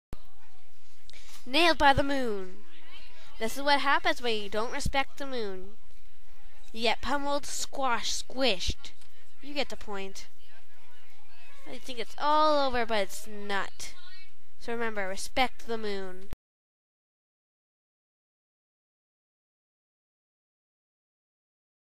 Nailed by Moon - Pivot Stickman Project
A student's video created by using Pivot Stickman and Windows Movie Maker.